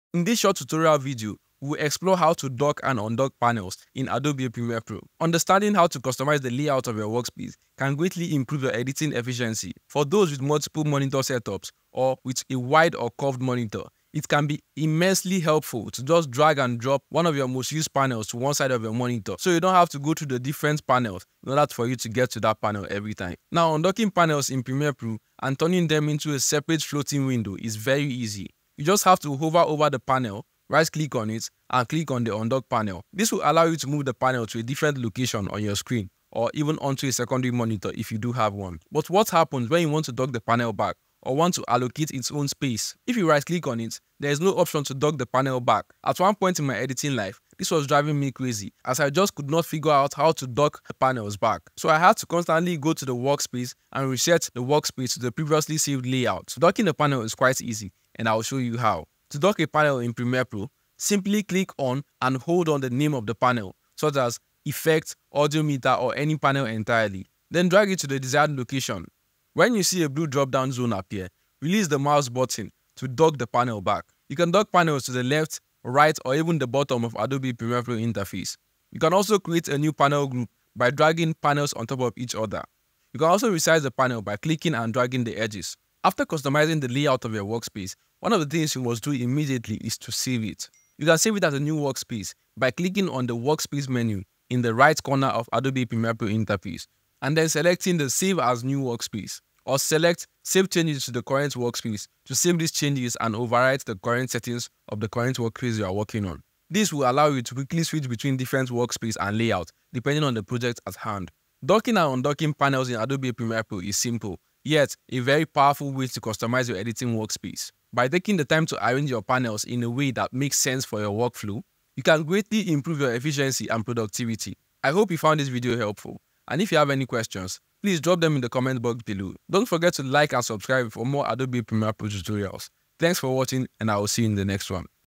How to Dock And Undock Panels In Premiere Pro
If you've ever found yourself overwhelmed by the default layout of Premiere Pro or simply want to customize your workspace for maximum efficiency, you're in the right place.

In this step-by-step guide, we delve deep into the art of docking and undocking panels within Adobe Premiere Pro. Whether you're a beginner eager to streamline your editing process or a seasoned professional looking for some advanced workspace customization, this tutorial caters to all.

Understanding the Workspace: A quick overview of Adobe Premiere Pro's default layout and the various panels at your disposal.

Docking Panels: Learn the techniques to rearrange and group panels to fit your specific editing needs. Discover how to lock panels into place for a seamless editing experience.

Undocking Panels: Dive into the flexibility of undocking panels to create a more fluid and personalized workspace. Master the techniques to move, resize, and float panels for optimal visibility and accessibility.

Tips & Tricks: Unlock pro tips on managing multiple panels, creating custom workspaces, and saving your preferred layouts for future projects.